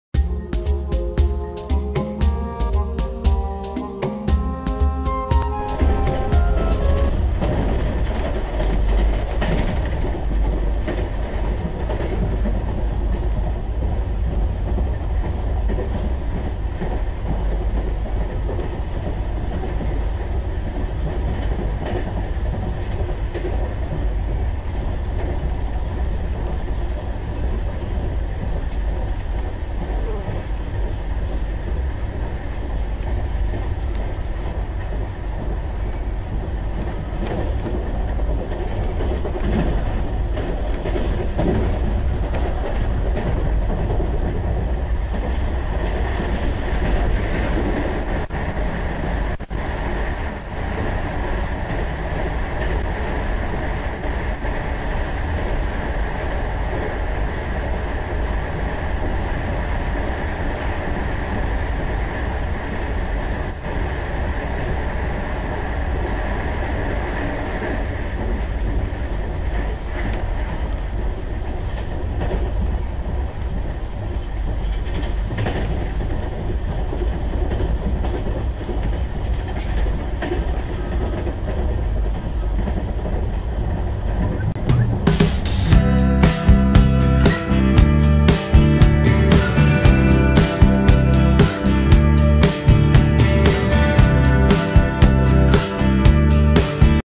"Rajdhani At It's Best": "ERODE 3D On Rampage": 12431 TVC Raj Skips Vilavade & A Tunnel In Fury!
Indian Railways: Konkan Railways: Central Railways: TVC Rajdhani On Rampage!

Erode (ED) WDM3D #11101 Hauled With 12431 Tiruvananthapuram- Nizamuddin Rajdhani Express Doing Her Best! Skips Vilavade & A Tunnel In Fury!

Enjoy The Speed Action!

Location:- Vilavade, Konkan Railways
Time:- 1.30pm
Camera:- Nikon Coolpix S7000

So,
Hope U Have Enjoyed The Video!